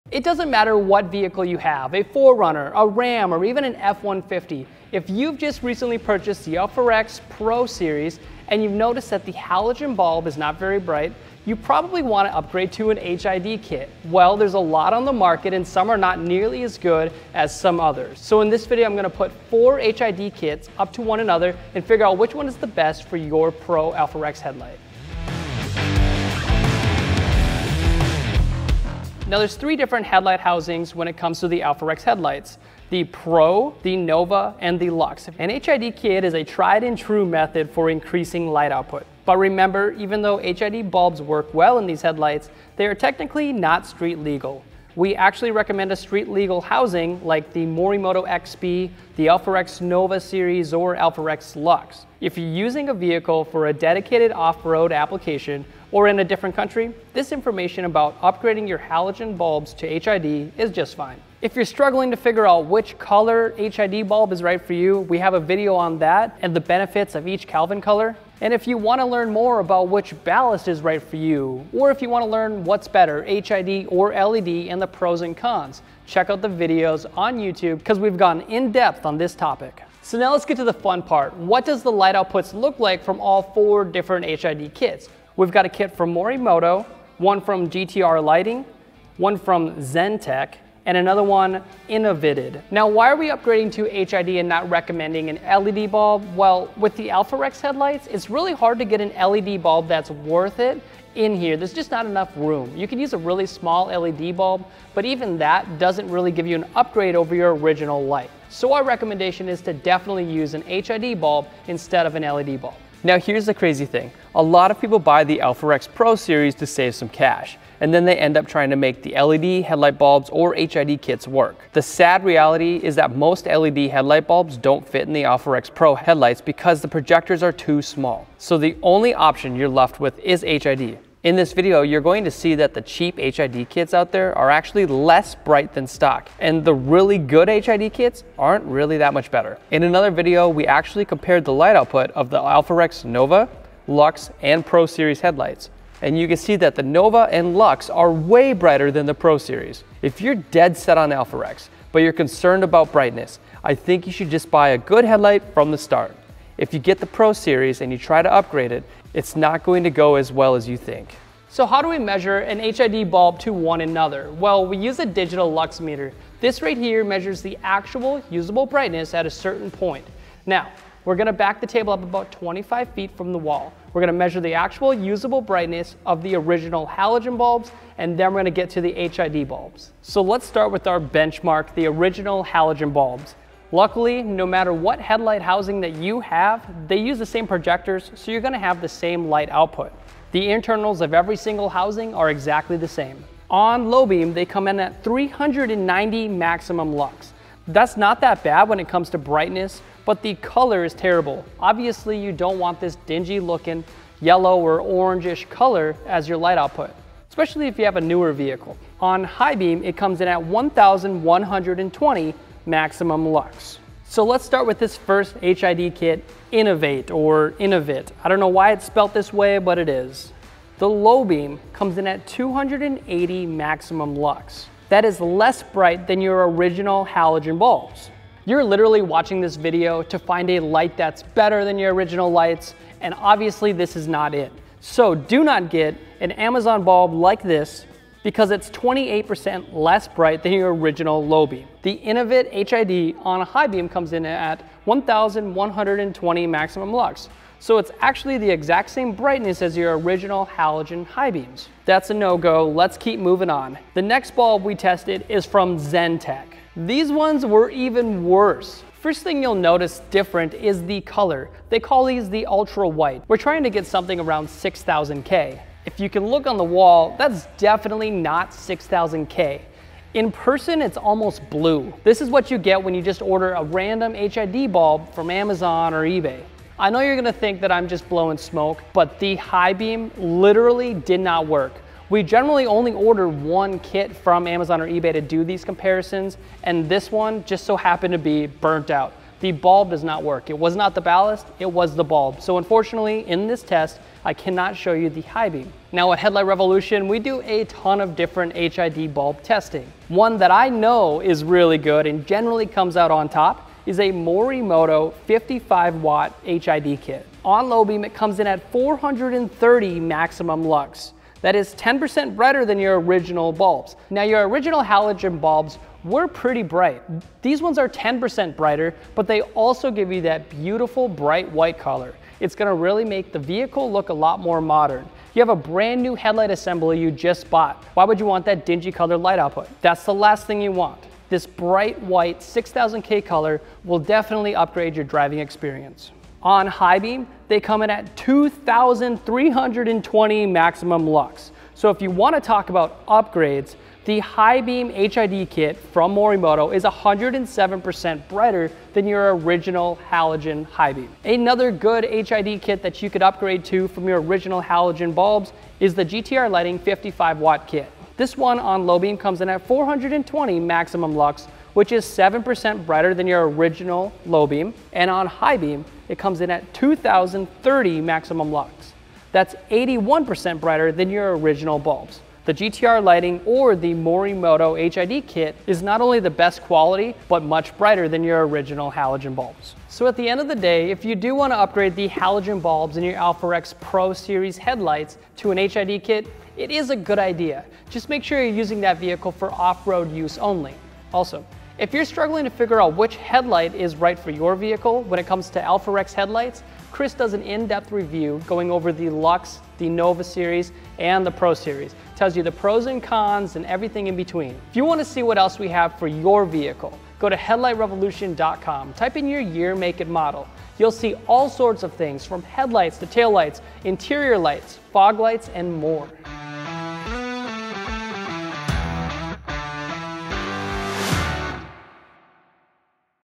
What is the best HID kit for your AlphaRex Pro Series? | Headlight Revolution Review
Top Rated HID KITs:
H7: Morimoto 50W HID Kit:

LED Replacement:
H7: Morimoto 2Stroke 3.0:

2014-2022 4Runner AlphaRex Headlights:
NOVA:

What is the best HID bulb for your dedicated off-road 4Runner? Here at Headlight Revolution, We Do Lighting Better, Because We Test! In today's video, Josh goes over 5 popular HID Kits and compares each of them to the Stock Halogen Light outputs. Tune in to see which is the best option for you to go with and why.

The AlphaRex PRO headlights are using H7 halogen bulbs for low beams and also H7 halogen bulbs for high beams.

(The headlights are using H7 halogen bulbs for low beams and also an H7 halogen bulb for high beams.)

00:00 - Intro: What is the best HID Upgrade?
00:29 - Different Alphrex Headlight Models
01:01 - Other Informative Lighting Videos
01:20 - The HID Kits we will be testing
01:33 - Why did we choose HID over LED?
02:22 - Alpharex Stock Light outputs (All 3 Models)
02:41 - How we measure HID bulbs to one another.
03:00 - Benchmark Light output test (Stock Halogen)
03:34 - Innovited HID Kit
04:16 - Xentec HID Kit
05:03 - Morimoto 55w HID Kit
05:54 - GTR Lighting 55W HID Kit
06:20 - Final Thoughts

⚡Low Beam Lux Numbers⚡
🔦 Stock Halogen Low Beam: 390 Max Lux
🔦 INNOVATE: 280 Max Lux (28% Less Brighter than stock)
🔦 XENTEC: 200 Max Lux (48% Less Brighter)
🔦 GTR Lighting 55W HID Kit: 420 Max Lux (7% Brighter than stock)
🔦 Morimoto 55W HID Kit: 430 Max Lux (10% Brighter)

⚡High Beam Lux Numbers⚡
🔦 Stock Halogen High Beam: 1120 Max Lux
🔦 INNOVATE: 1120 Max Lux (Same as OEM Halogen)
🔦 XENTEC: Unsure bulb came in broken
🔦 GTR Lighting 55W HID Kit: 2030 Max Lux (81% Brighter than stock)
🔦 Morimoto 55W HID Kit: 2320 Max Lux (107% Brighter)

For the best LED automotive lighting selection in the world, check us out

We Do Lighting Better Because We Test!